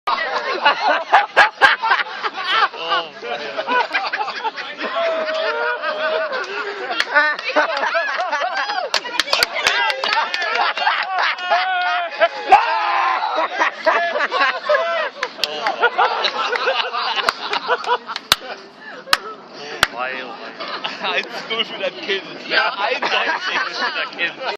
Karsten Beck Kinderschreck
Karsten Beck hat vor keinem Gegner Angst!

Fand bei GHW´s Beer, BBQ and Bash statt. Alle Infos zur GHW unter

Nächste Show am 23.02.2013 in Klein Gerau u.a. mit dem WWE Hall of Famer Ted DiBiase!